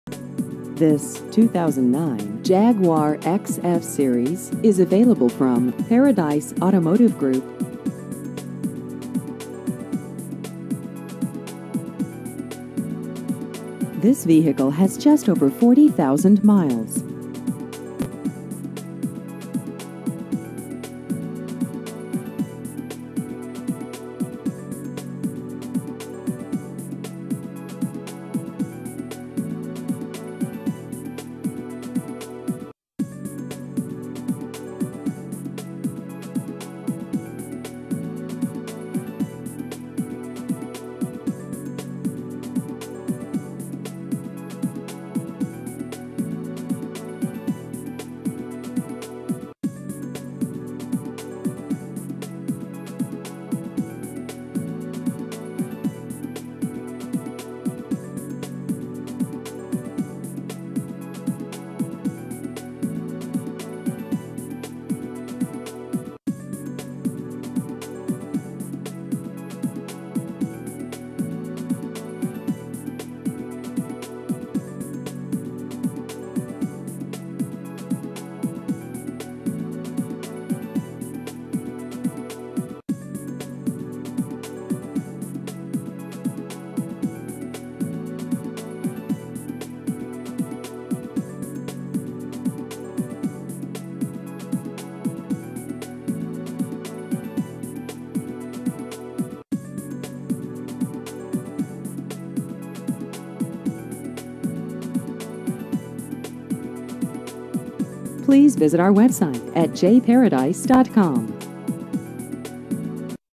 2009 Jaguar XF-Series Used Cars San Juan Capistrano CA
This 2009 Jaguar XF-Series is available from Paradise Automotive Group.